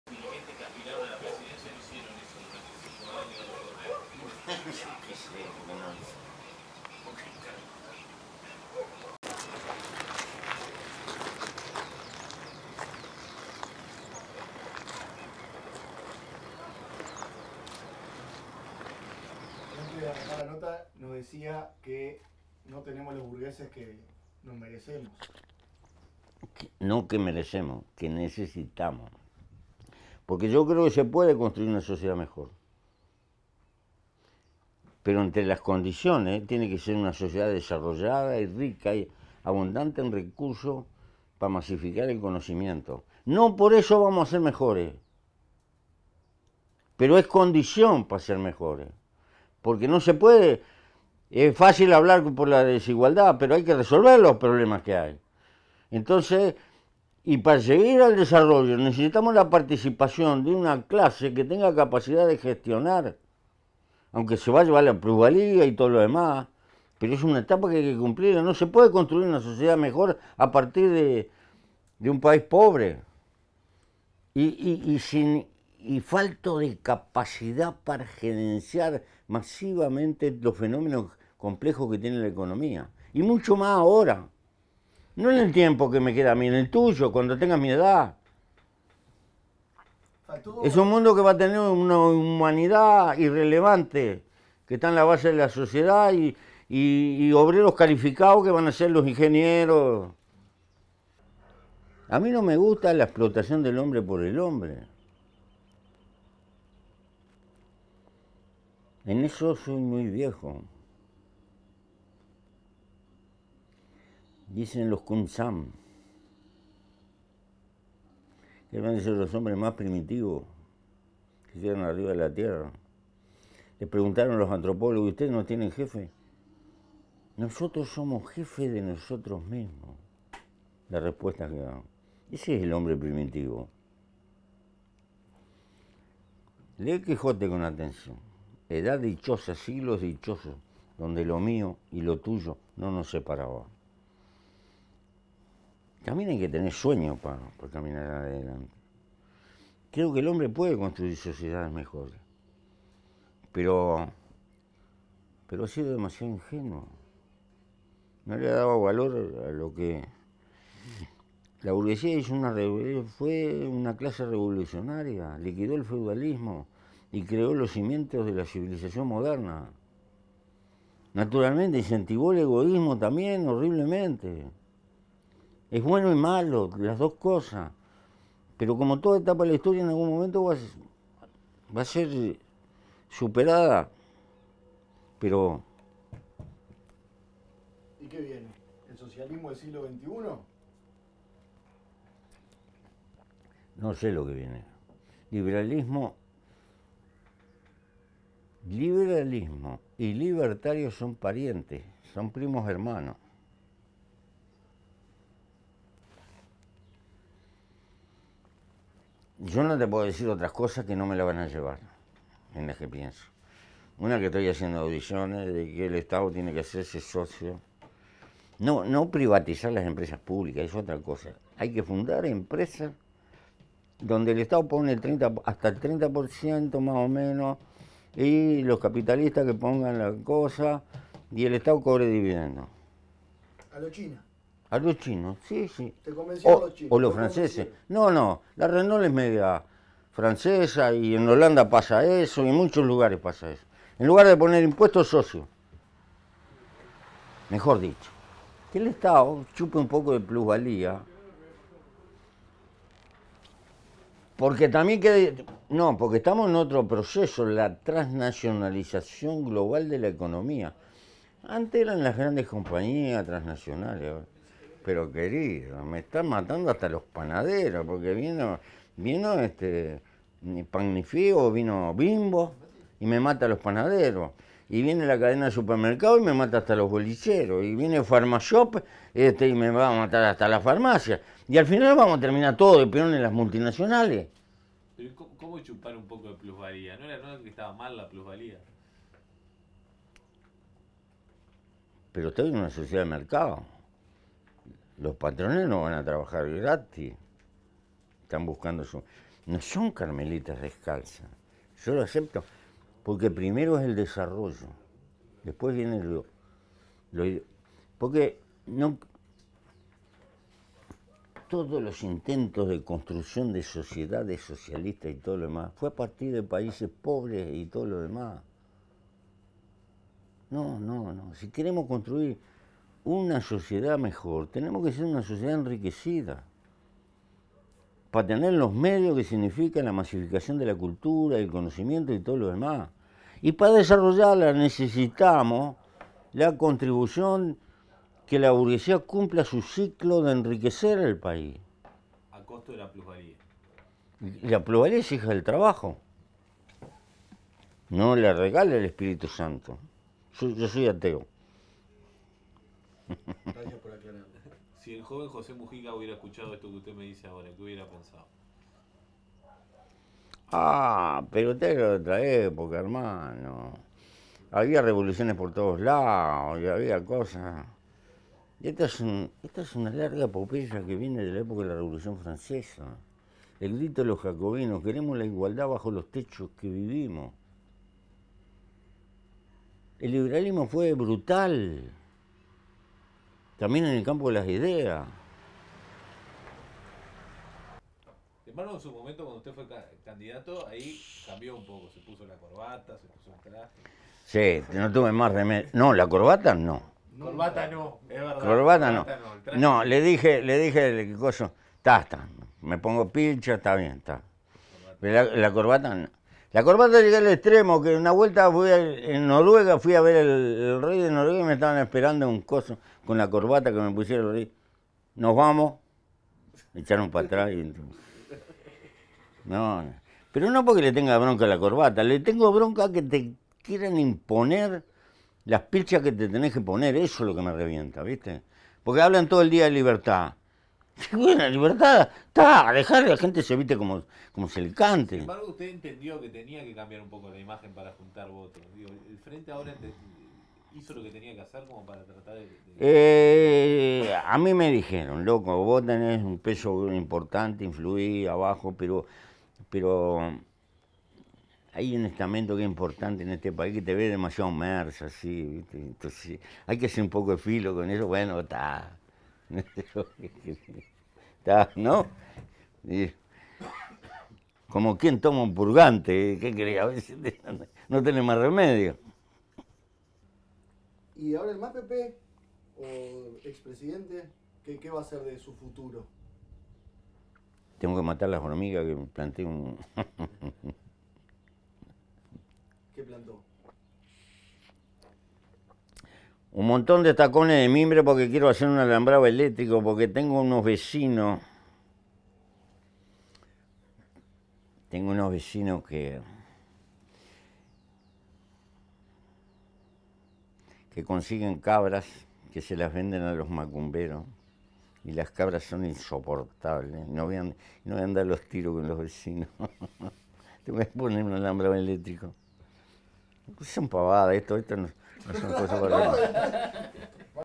José Mujica: "No tenemos la burguesía que necesitamos"
El expresidente de Uruguay, Pepe Mujica, dejará de ser Senador el 20 de octubre de 2020 debido a la pandemia de coronavirus COVID-19, ya que forma parte del grupo de riesgo. Explicó su filosofía de vida, el capitalismo y la idea de forjar una sociedad mejor.

"No tenemos la burguesía que necesitamos. Creo que se puede construir una sociedad mejor pero entre las condiciones, tiene que ser una sociedad desarrollada y rica para masificar el conocimiento", afirmó.

También habla sobre el rol del Estado y la posibildad de realizar empresas en conjunto con empresas privadas, una idea que afirma que se inspiró en China y Francia. "Que el Estado chupe un poco de plusvalía", declaró.